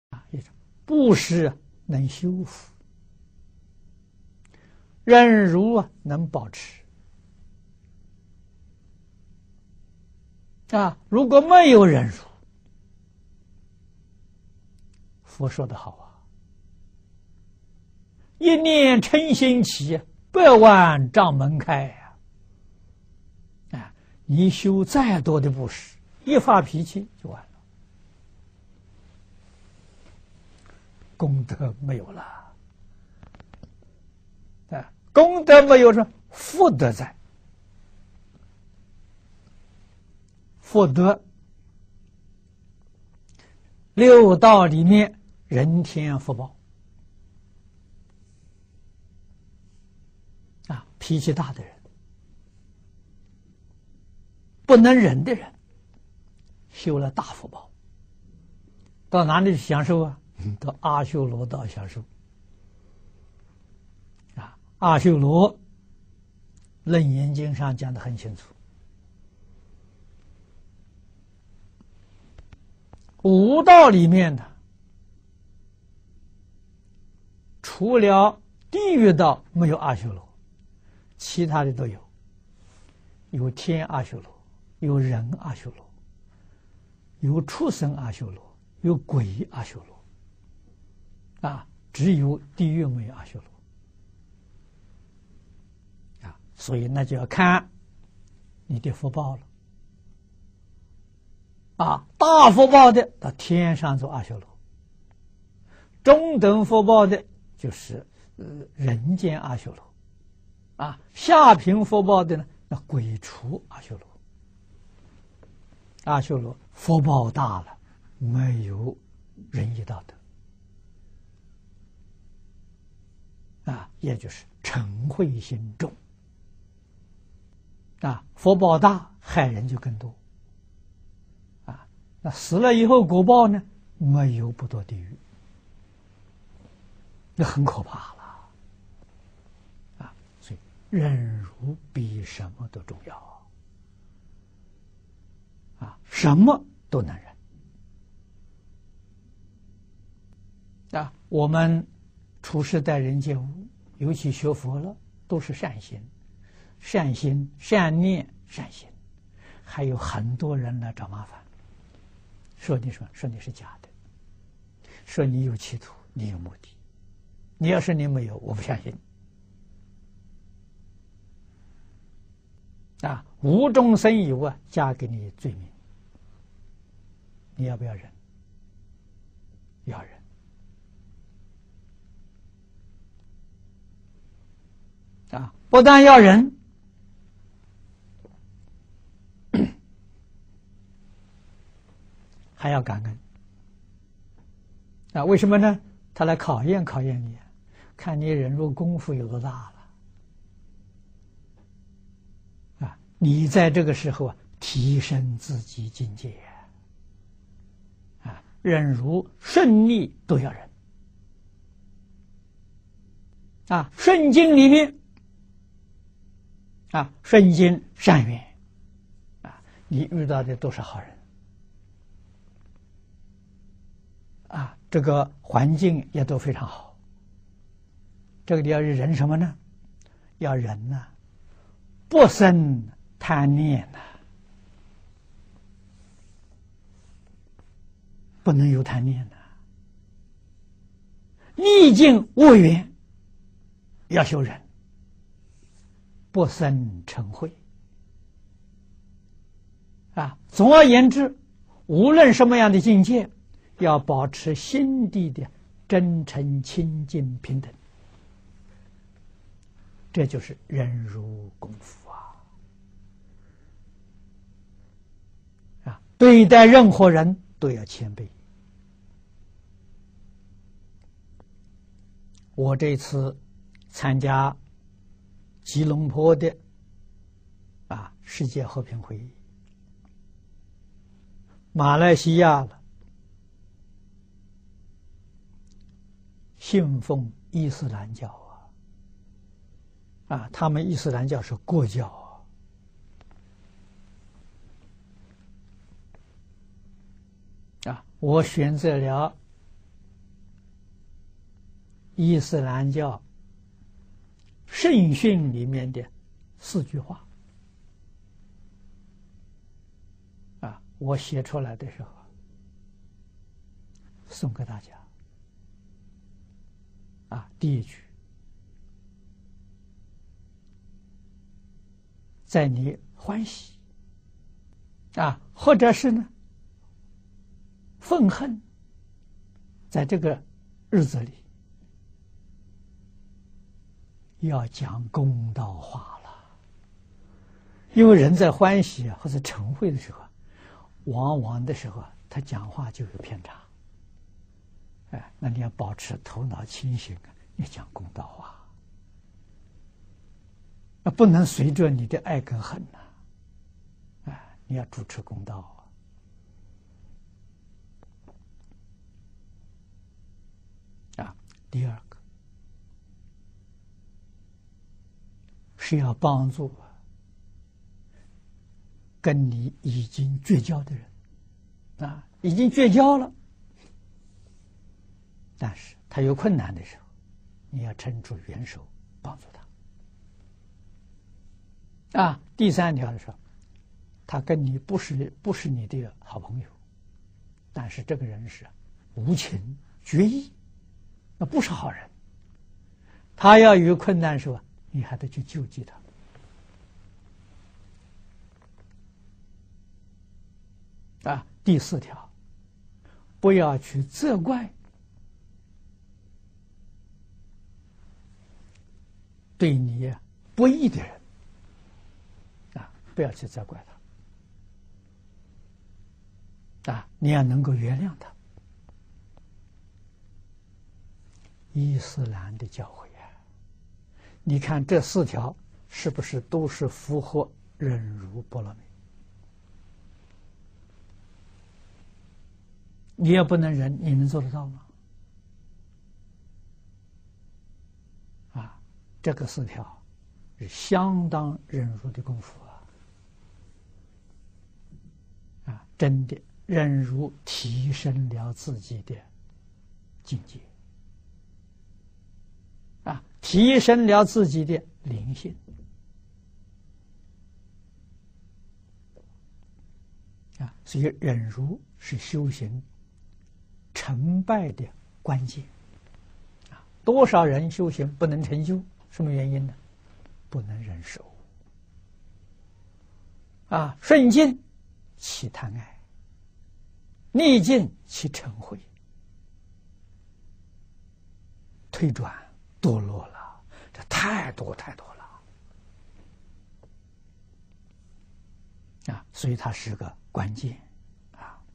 Master Chin Kung - Those Who are Generous but Grumpy will become Asuras 脾氣大不能忍修大福報到阿修羅道享受
(English subtitles) Master Chin-Kung (淨空法師, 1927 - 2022) talk dharma.
脾氣大不能忍修大福報到阿修羅道享受 Those Who are Generous but Grumpy will become Asuras
Subtitles:
Generosity can cultivate good fortune. Forbearance can maintain good fortune. Without forbearance, Buddha said it well: One thought of grudge, will open a hundred doors of the barrier. No matter how much giving you practice, once you lose temper, your merit will be gone. The merit is gone, but the good fortune is still there. If you have good fortune, you may enjoy that in the realms of human and celestial. Those who are grumpy and can't bear humiliation have cultivated great fortune. Where to enjoy it?
Go to asura realm to enjoy. Asura, the Shurangama Sutra makes it very clear. Except for the realm of hell, there are asuras in the other realms. There are celestial asuras, human asuras. There are animal asuras, and ghost asuras. Only in hell there are no asuras. Then it depends on your good fortune. With great fortune, you go to heaven as an asura. With medium good fortune, you are an asura in the human world. With less good fortune, you are asuras of ghost and animal realms. Though asuras are blessed with good fortune, they are neither kind nor righteous, and are very grumpy.
2:00
The greater their good fortune, the more beings they can harm. After death, they usually end up in hell as retribution, that is terrible. So forbearance is the most important, and you have to bear everything. To deal with things and get along with people, especially we Buddhists should have a big heart and do good deeds. But many people look for trouble. What do they say ? Say you are fake with intentions and purposes. You say you are not. They say: “I don't believe.” Make up facts out of thin air and add a crime on you.
Will you bear it? Yes, you should bear it. Not only do you have to endure but you also have to be grateful. Why ? He came to give you a test to see how well you could endure humiliation. You can upgrade yourself at this time. Whether you are in good times or bad, you have to endure. When you are in a good time and good conditions, The people you meet are all good and the environment is also good. What are you going to endure? Avoid greed or obsession. You shouldn't be obsessed.
4:00
In bad times and bad conditions, you must endure without resentment. All in all, no matter what state you are in, keep your heart sincere, pure, and equal. This is the ability of forbearance. Treat everyone with humility.
I attended the World Peace Conference in Kuala Lumpur, Malaysia this time (2005). Malaysian believes in Islam and Islam is their state religion. I chose 4 sentences from the Hadith of Islam. I wrote them out for everyone.
The 1st sentence, in the day of your joy or resentment, You should speak fair words. It is because when people are joyful or angry,  they tend to speak with bias, must keep their heads above water and speak fairly. You can't go along with your love and hate. You must uphold justice.
6:00
2nd, help people who have broken up with you, though the relation is broken off. But when he is in trouble, you may lend a helping hand.
3rd, he is not your good friend. He is ruthless and unjust, and is a villain. But when he is in trouble and helpless, you have to give him alms.
4th, do not blame the person who is unjust to you. Do not blame him, and forgive him.
The teaching of Islam. Are the 4 items in line with the perfection of forbearance? If you can't stand it, can you do it? You must have an excellent forbearance ability to do these 4 items. Really, forbearance improves your own state and enhance your spirituality. Therefore, forbearance is the key to the success of practice. So many people fail to achieve success in practice. Why? They can not bear. Prosperity breeds greed, adversity breeds hatred. Then they retreat and deprave. Too many cases. So it's a key.